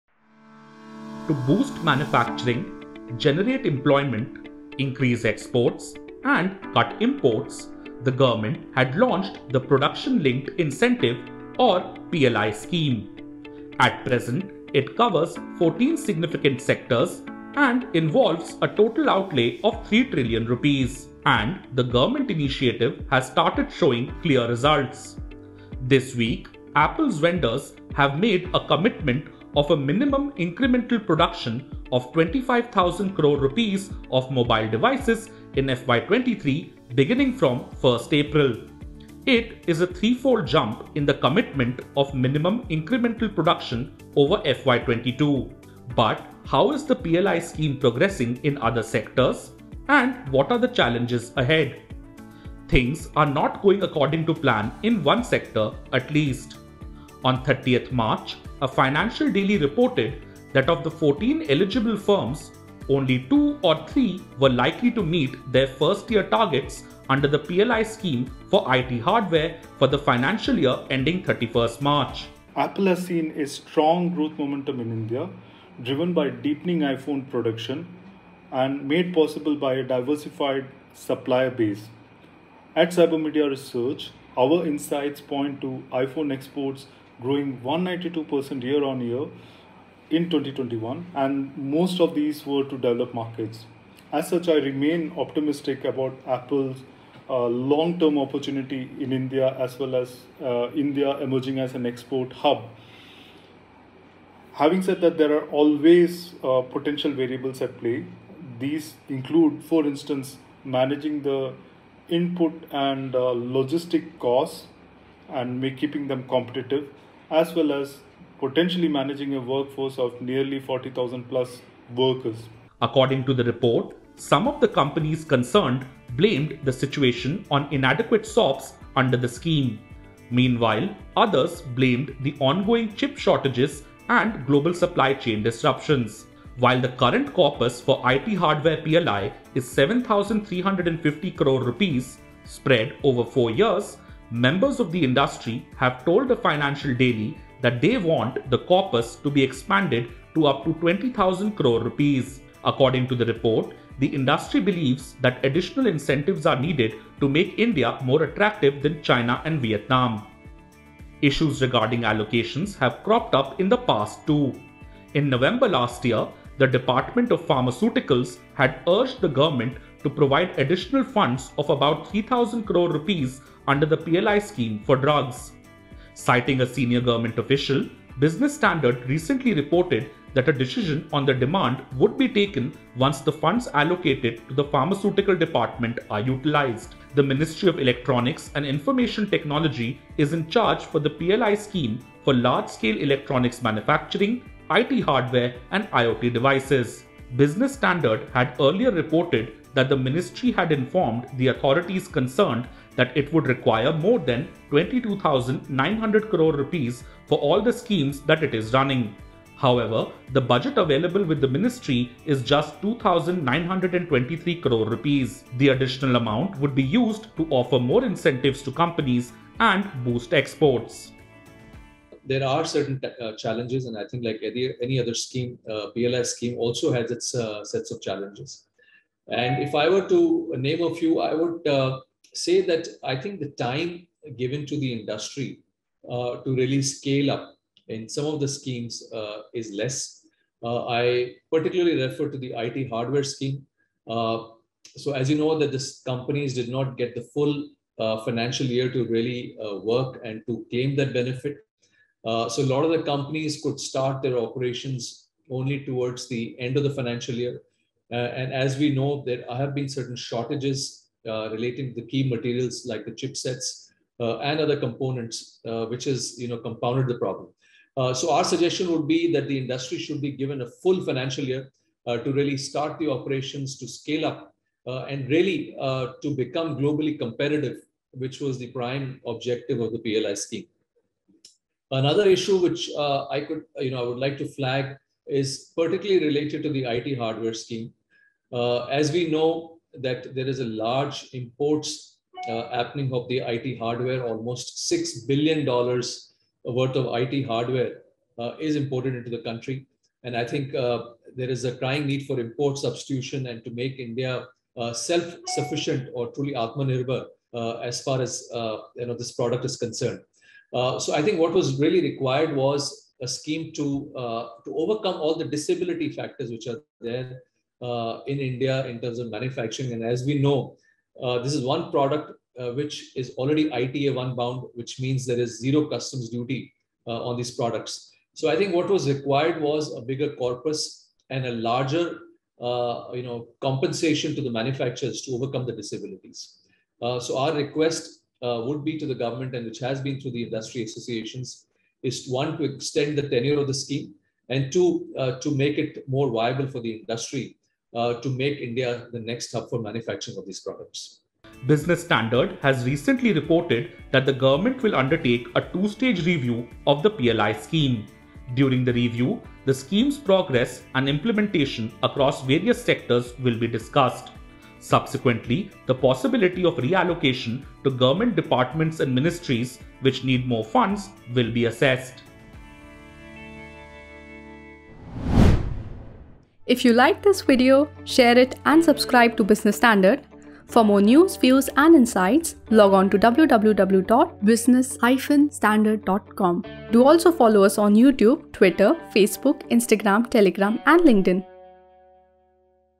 What are the challenges facing the PLI scheme?
Apple is ramping up its production in India. The govt’s production-linked incentive, or PLI scheme, seems to have helped it. Is it good for other sectors too? And what are the challenges it is facing?